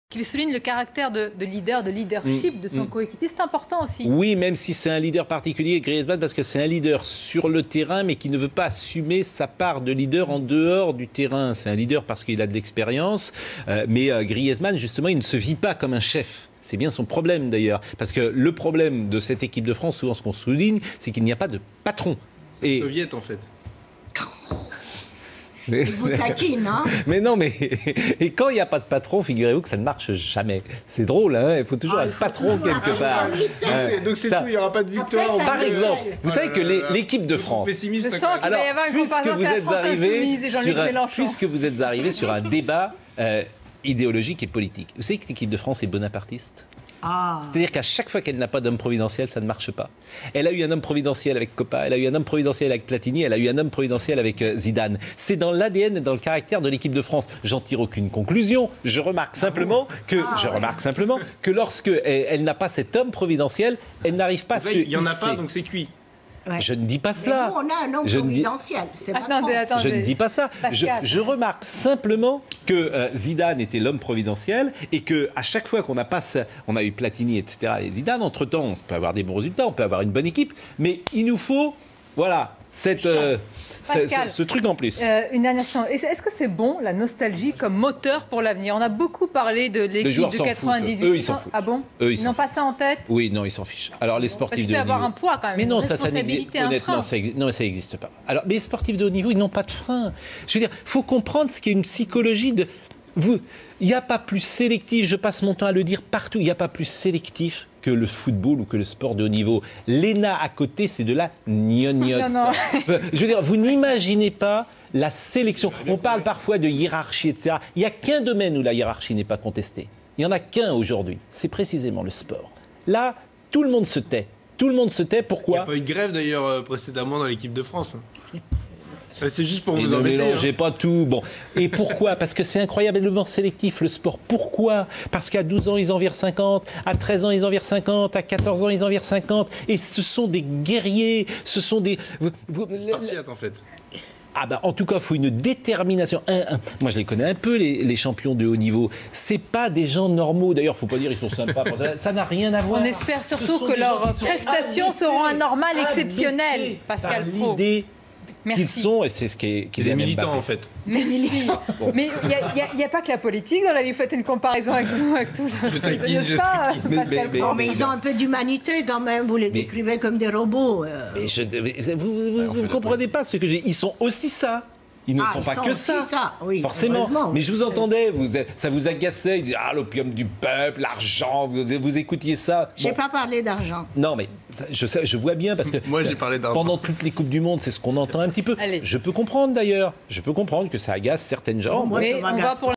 Foot, politique et bonapartisme : Pascal Praud en roue libre sur Cnews
Quand Pascal Praud s'emballe, Esther Benbassa et Ugo Bernalicis le taquine.